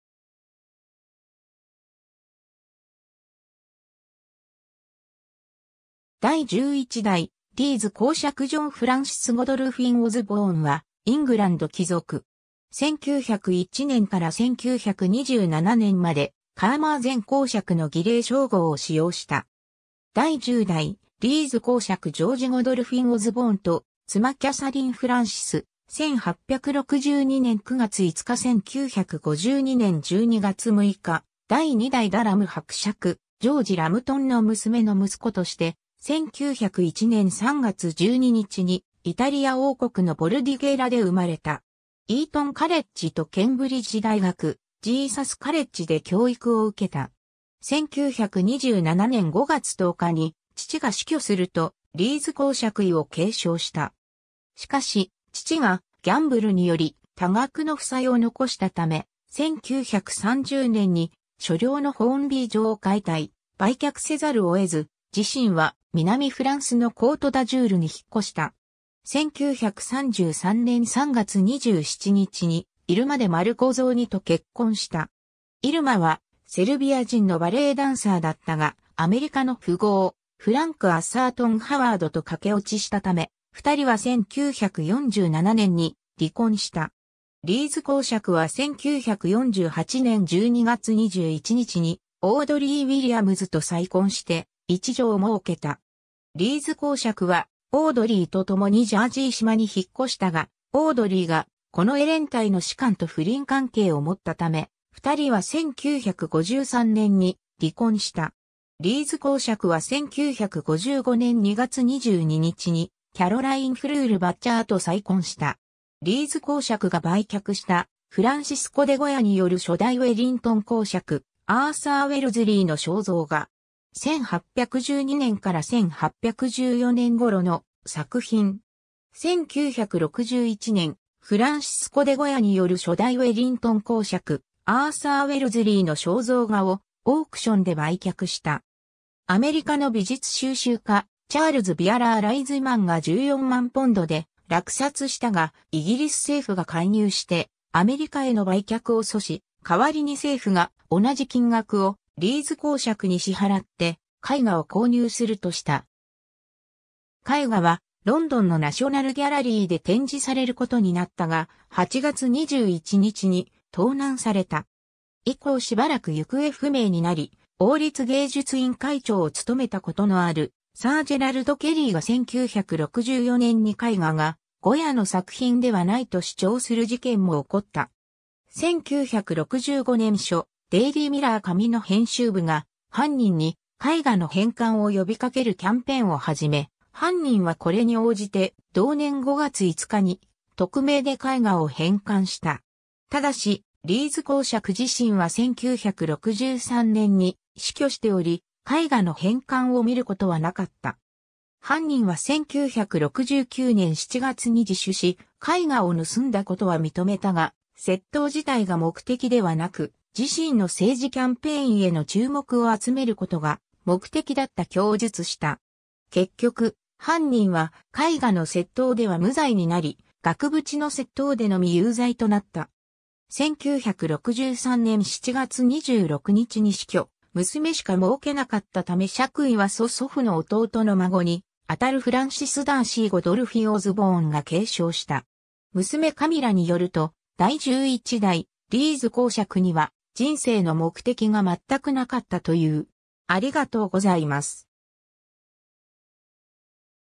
ジョン・オズボーン (第11代リーズ公爵)
第11代リーズ公爵ジョン・フランシス・ゴドルフィン・オズボーン（英語: John Francis Godolphin Osborne、1901年3月12日 – 1963年7月26日）は、イングランド貴族。
1901年から1927年までカーマーゼン侯爵の儀礼称号を使用した。
 第10代リーズ公爵ジョージ・ゴドルフィン・オズボーンと妻キャサリン・フランシス（Katherine Frances、旧姓ラムトン（Lambton）、1862年9月5日 – 1952年12月6日、第2代ダラム伯爵ジョージ・ラムトンの娘）の息子として、1901年3月12日にイタリア王国のボルディゲーラで生まれた。
イートン・カレッジとケンブリッジ大学ジーサス・カレッジで教育を受けた。
 1927年5月10日に父が死去すると、リーズ公爵位を継承した。
しかし、父がギャンブルにより多額の負債を残したため、1930年に所領のホーンビー城を解体・売却せざるを得ず、自身は南フランスのコート・ダジュールに引っ越した。
 1933年3月27日にイルマ・デ・マルコゾーニと結婚した。
イルマはセルビア人のバレーダンサーだったが、アメリカの富豪フランク・アサートン・ハワード（Frank Atherton Howard）と駆け落ちしたため、2人は1947年に離婚した。
リーズ公爵は1948年12月21日にオードリー・ウィリアムズ（Audrey Williams、アーサー・エヴァンス・ベッドワード・ウィリアムズの元妻、デズモンド・ヤングの娘）と再婚して、1女をもうけた。
 リーズ公爵はオードリーとともにジャージー島に引っ越したが、オードリーが近衛連隊の士官と不倫関係をもったため、2人は1953年に離婚した。
リーズ公爵は1955年2月22日にキャロライン・フルール・ヴァッチャー（2005年7月16日没）と再婚した。
 リーズ公爵が売却した、フランシスコ・デ・ゴヤによる初代ウェリントン公爵アーサー・ウェルズリーの肖像画。
1812年から1814年ごろの作品。
 1961年、フランシスコ・デ・ゴヤによる初代ウェリントン公爵アーサー・ウェルズリーの肖像画をオークションで売却した。
アメリカの美術収集家チャールズ・ビアラー・ライズマンが14万ポンドで落札したが、イギリス政府が介入してアメリカへの売却を阻止、代わりに政府が同じ金額をリーズ公爵に支払って絵画を購入するとした。
絵画はロンドンのナショナル・ギャラリーで展示されることになったが、8月21日に盗難された。
以降しばらく行方不明になり、王立芸術院会長を務めたことのあるサー・ジェラルド・ケリーが1964年に絵画がゴヤの作品ではないと主張する事件も起こった。
1965年初、『デイリー・ミラー』紙の編集部が犯人に絵画の返還を呼びかけるキャンペーンをはじめ、犯人はこれに応じて同年5月5日に匿名で絵画を返還した。
ただし、リーズ公爵自身は1963年に死去しており、絵画の返還を見ることはなかった。
犯人は1969年7月に自首し、絵画を盗んだことは認めたが、窃盗自体が目的ではなく、自身の政治キャンペーンへの注目を集めることが目的だった供述した。
結局、犯人は絵画の窃盗では無罪になり、額縁の窃盗でのみ有罪となった。
 1963年7月26日に死去、娘しかもうけなかったため爵位は曽祖父の弟の孫にあたるフランシス・ダーシー・ゴドルフィン・オズボーンが継承した。
 娘カミラによると、第11代リーズ公爵には「人生の目的が全くなかった」（He had absolutely no purpose in life）という。